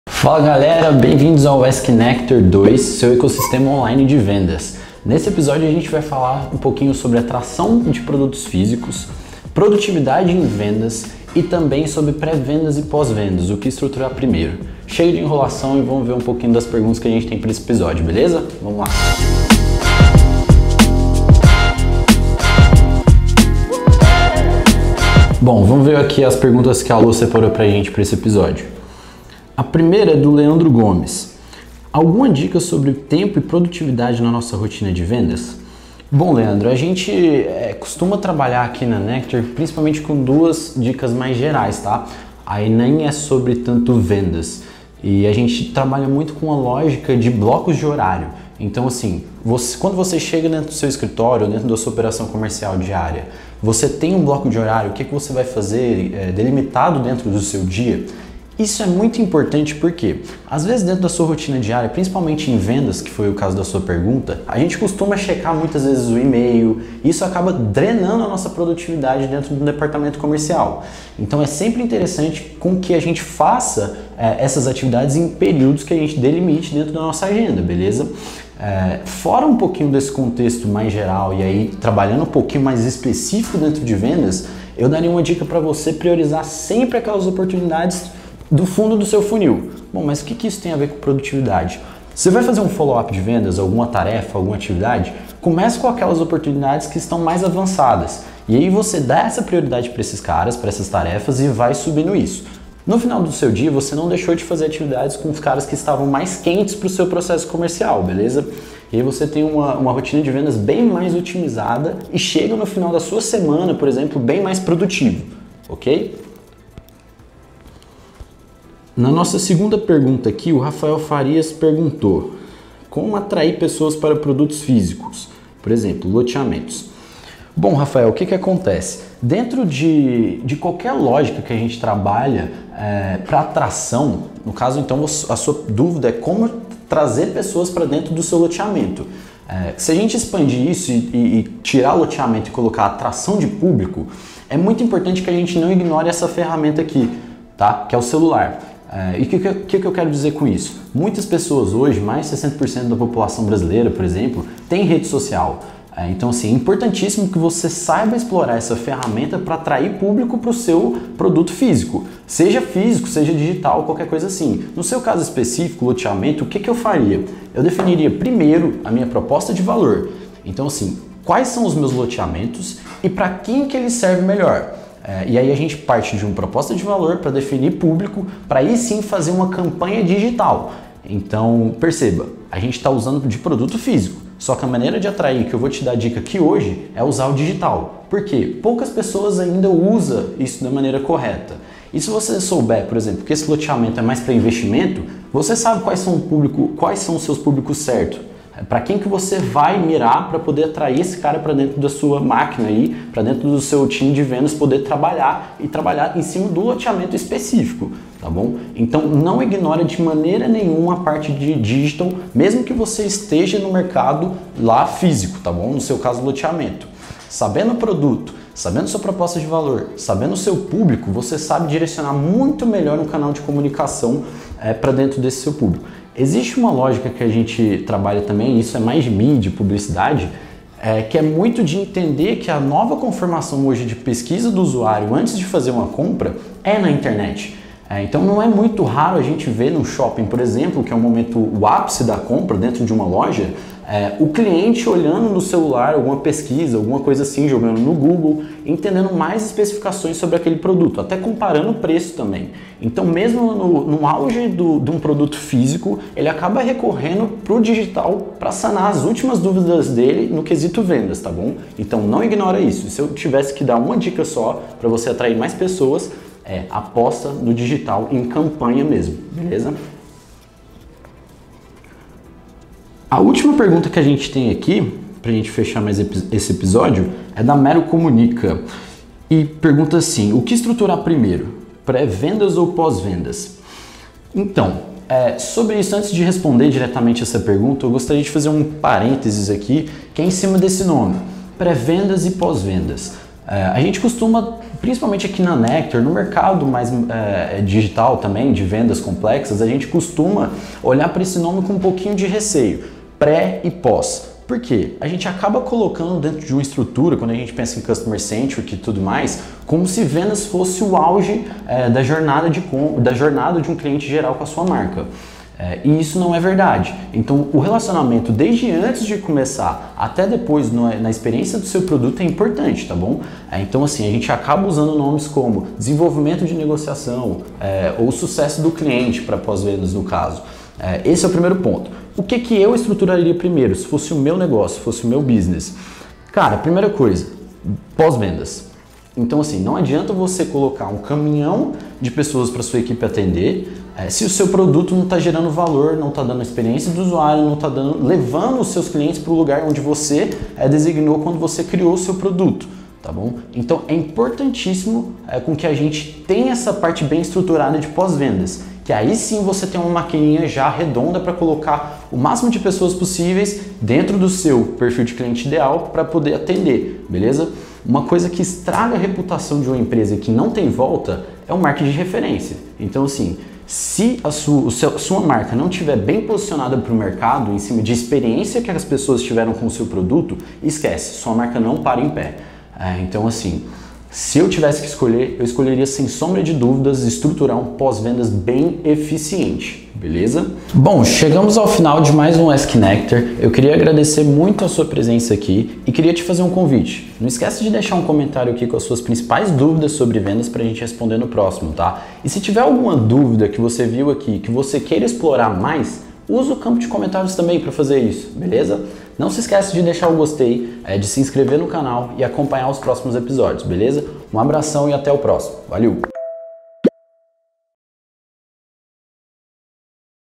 EP.02 - PRODUTIVIDADE, ATRAÇÃO PARA PRODUTOS FÍSICOS E PRIORIDADE NA CONSTRUÇÃO DE DPTOS
Nesse segundo episódio falamos sobre produtividade, atração para produtos físicos e prioridade na construção de departamentos.

/// Perguntas

0:33 1. Alguma dica sobre tempo e produtividade na nossa rotina de vendas?
2:03 2. Como atrair pessoas para produtos físicos, por exemplo, loteamentos
4:40 3. O que estruturar primeiro? Pré vendas ou pós vendas?

///Tem alguma dúvida?

Deixe nos comentários que vamos responder no próximo vídeo!

🏆 Quer receber conteúdos como esse?

/// Video creator